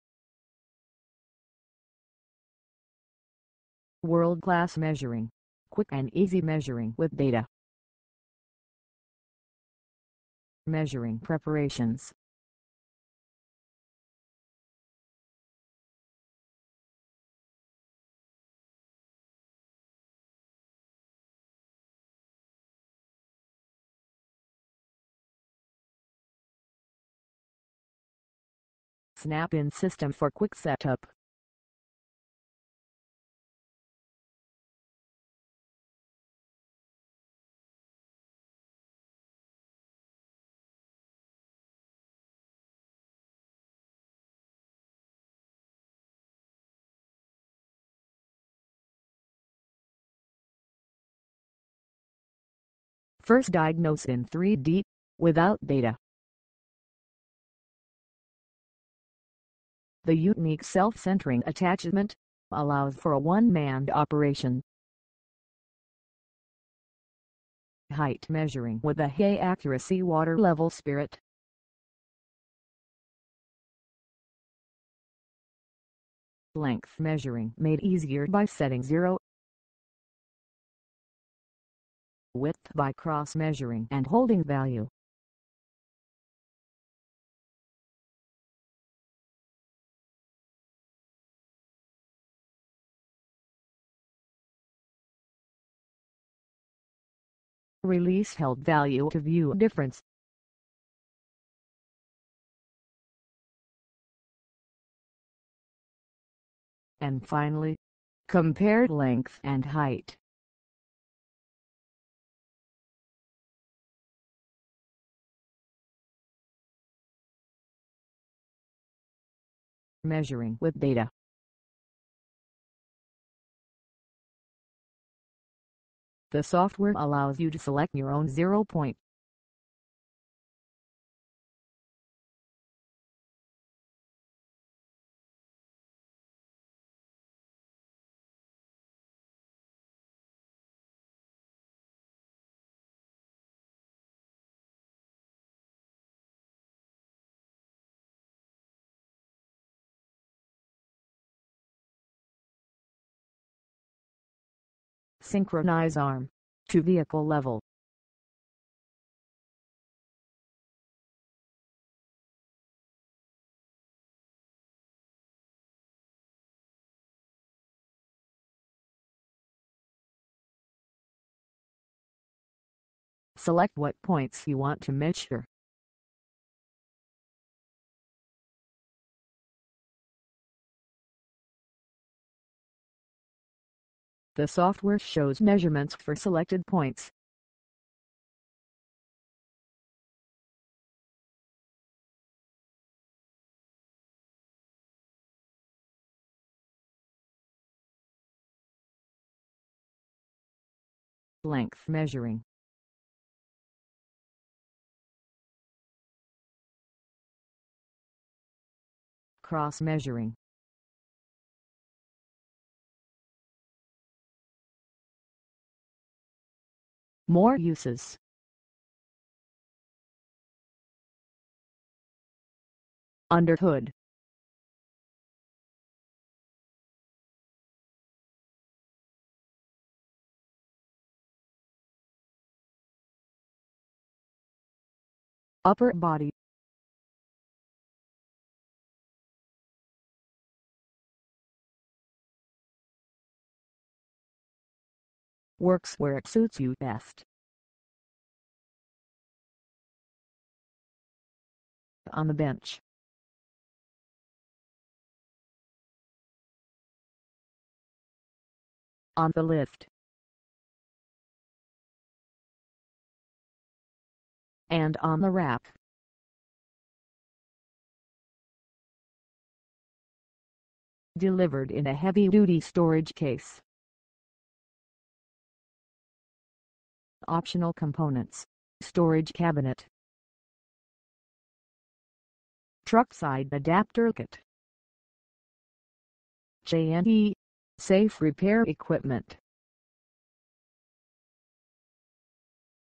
Мерительная система Allvis JNE
Измерительная линейка JNE Allvis предназначена для мерения геометрии кузова с доступом к интернет базе размеров автомобилей
Купить оборудование для автосервиса и шиномонтажа можно в компании СТО КОМПЛЕКТ-СЕРВИС ( СКС ),
телефоны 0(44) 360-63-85; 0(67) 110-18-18; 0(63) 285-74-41